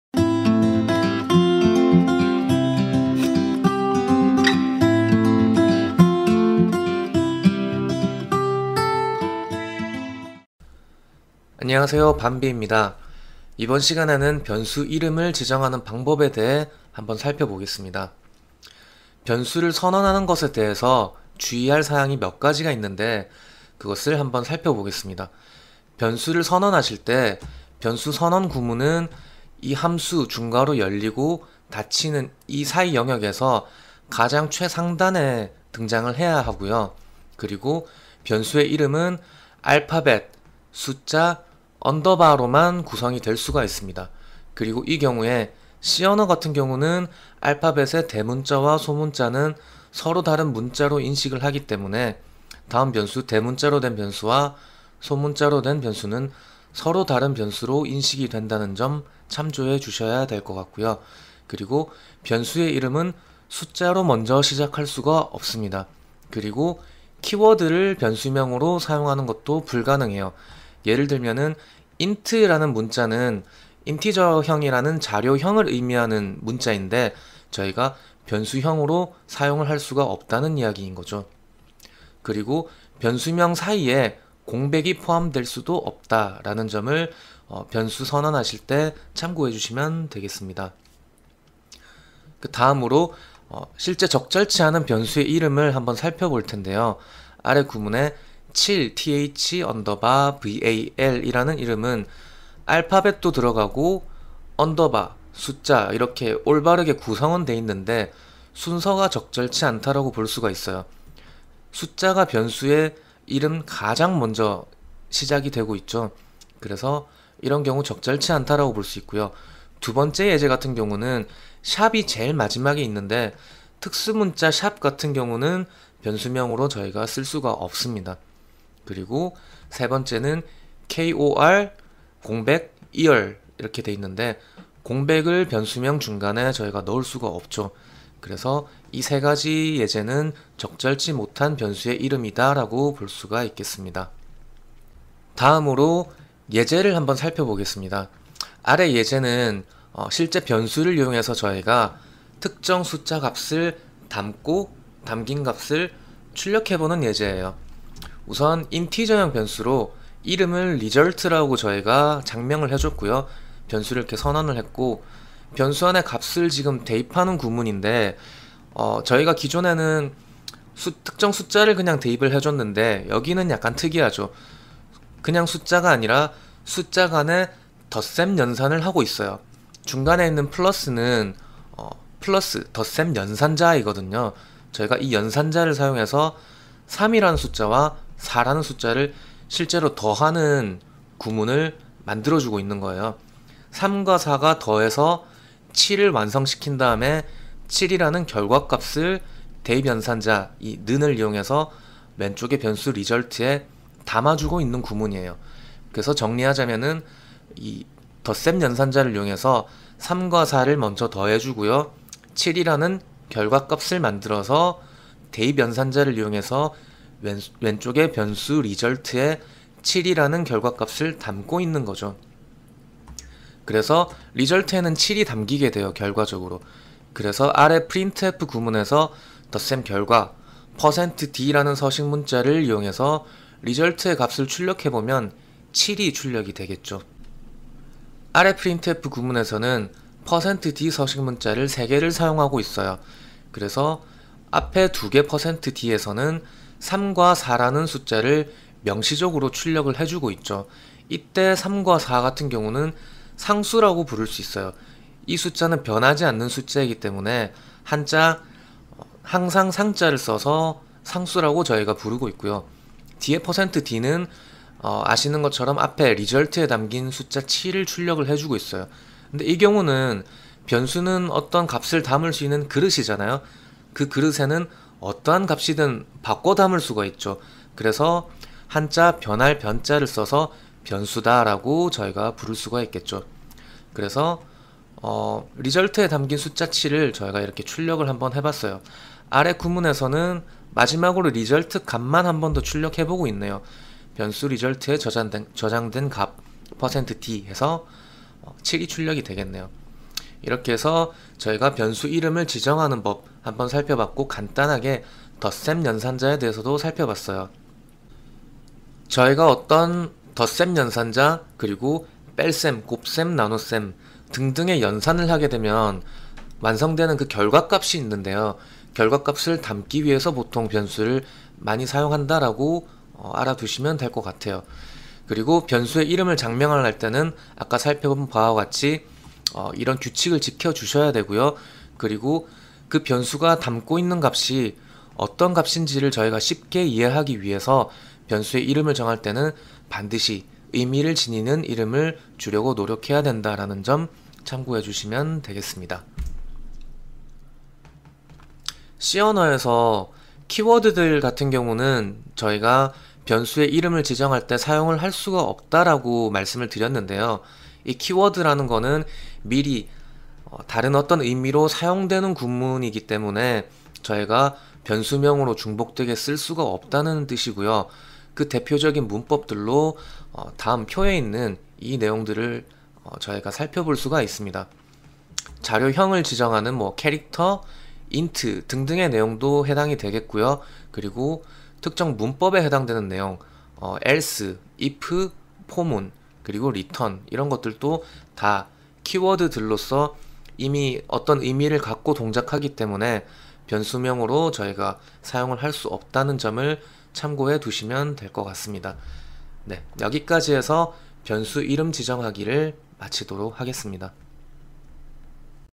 [C입문 #7] 변수 이름 지정하기 ★밤비와 코딩
C언어에서 변수의 이름을 지정하는 규칙에 대해 알아봅니다.

- 밤비와 코딩 네이버 카페(학습자료, 커뮤니티)

- 밤비와 코딩 아프리카 방송국(문제풀기, 질의응답)